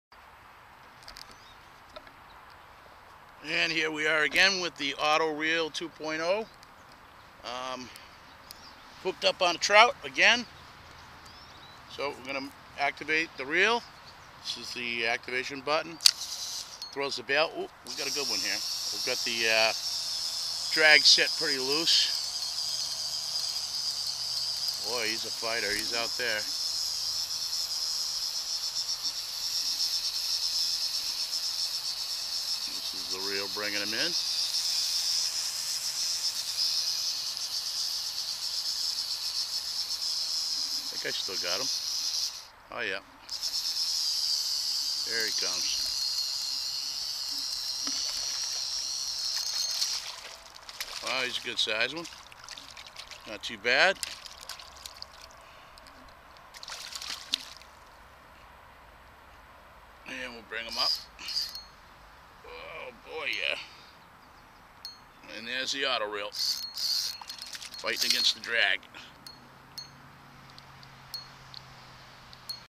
Quantum Hypercast HPL20 AUTO REEL 2.0
Invention for sale
Introducing the
AUTO REEL 2.0
now seeking funding to refine and improve this idea.

Designed for sports persons with a missing limb or hand related deficit.
This is a single handed fishing system. It can be used with one hand to cast and retrieve.
This is an invention I came up with 5 years ago.
This is an electric assisted fishing reel with the pole included.
I designed and assembled this myself. It's a geared planetary transmission motor,
Using a cog and belt design.
Attached to the Quantum Hypercast HPL20 3 bearing fresh water med tackle reel.
This system uses the snap shot 1 finger firing pin to cast and the power assist 1 finger / thumb switch to retrieve over 6 pounds. Its self contained, works flawlessly comes with a 14 volt dc Ni-cad power pack that's rechargeable, (uses a simple toy car charger to charge the batteries) not included.
Yes there are other power assisted reels out there, but most are made for heavy or large deep sea applications. And are very expensive.  This invention comes with some hand drawings and a parts list. The invention can be modified and or improved, also decompiled for blue-printing.  This is not patented. But can be. You will own all intellectual rights. This is a great investment opportunity.